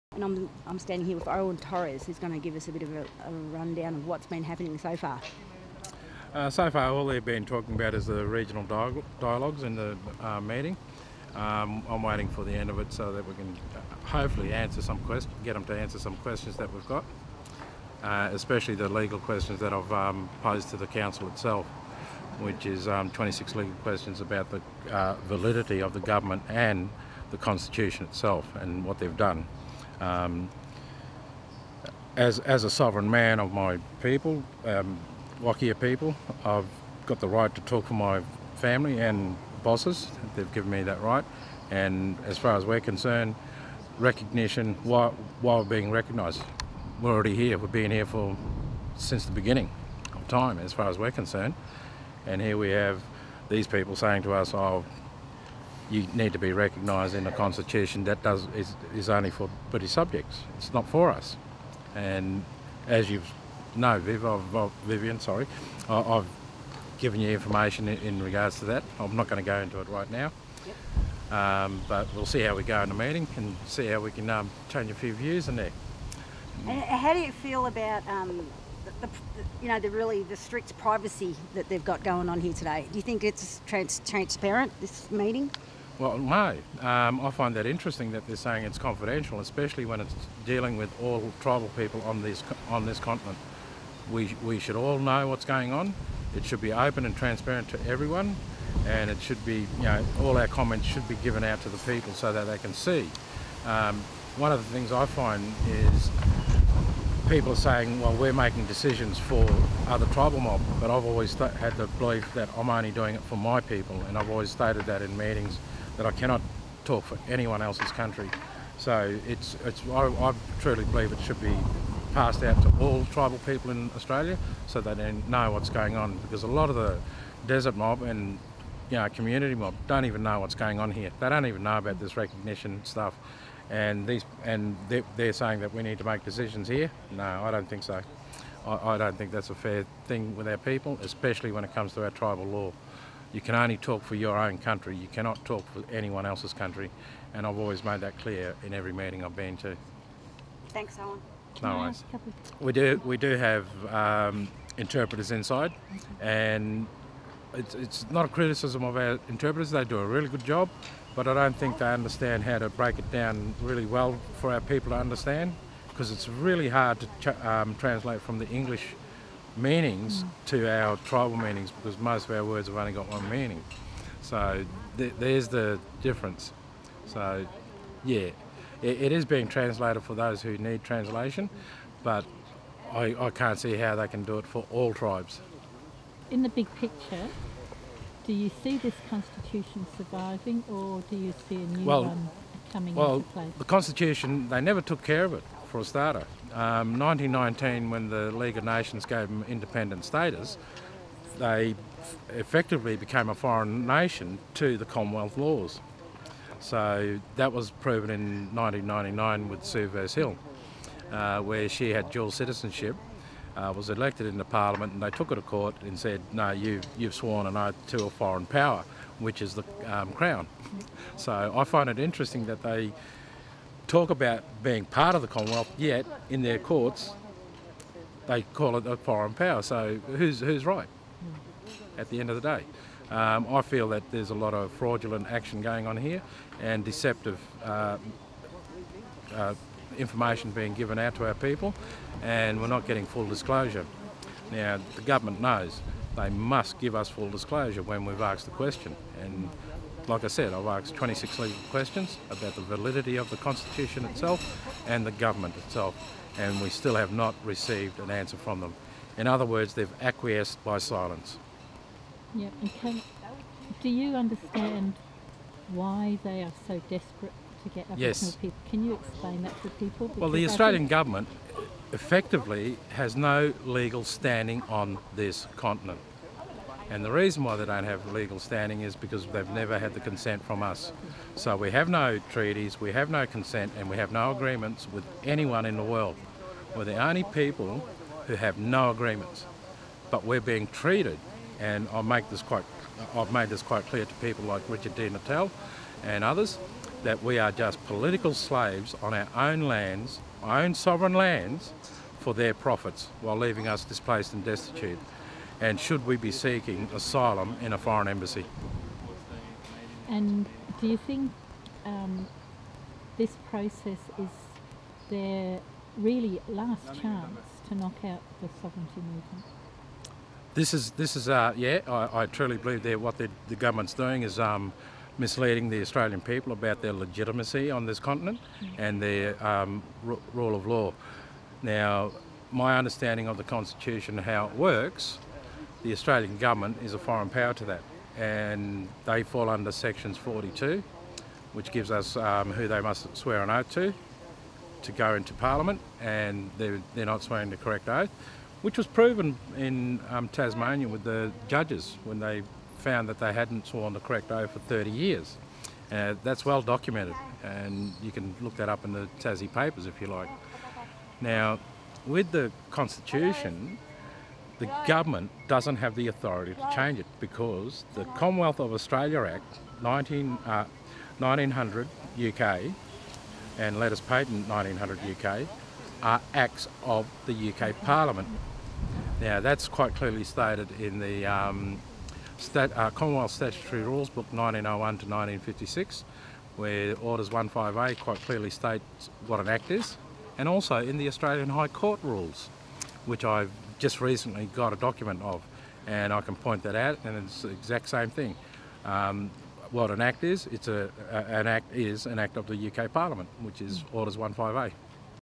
(6of20) Illegal Constitution from Britain -Owen Torres@Referendum Council, Yulara, NT
Owen Torres articulates the growing understanding that the Commonwealth of Australia does not have legal standing in the international community of Nations.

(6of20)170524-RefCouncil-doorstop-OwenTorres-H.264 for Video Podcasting